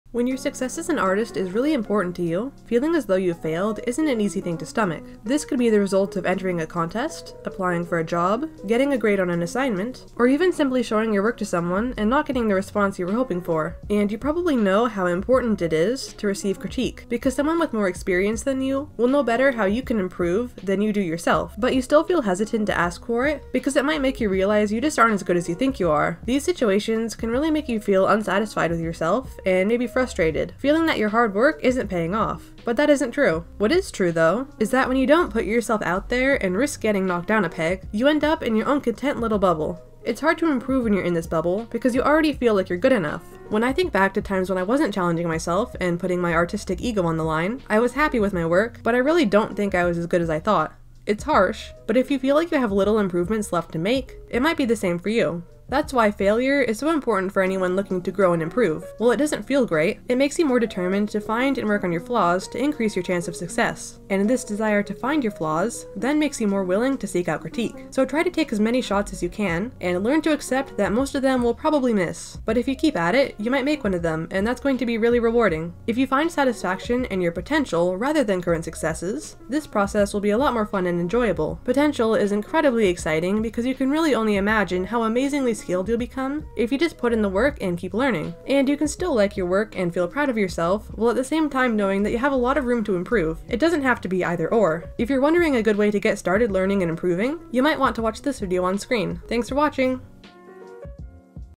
The importance of failure and critique for artists
When your success as an artist is really important to you, feeling as though you’ve failed isn’t an easy thing to stomach. This could be the result of entering a contest, applying for a job, getting a grade on an assignment, or even simply showing your work to someone and not getting the response you were hoping for. But believe it or not, this can be really good for you!

The more dots added, the more the stipple drawing will be revealed!

NOTE: It is very possible that topics I cover on this channel have been covered on other channels with similar ideas and terms, however I will never intentionally copy anyone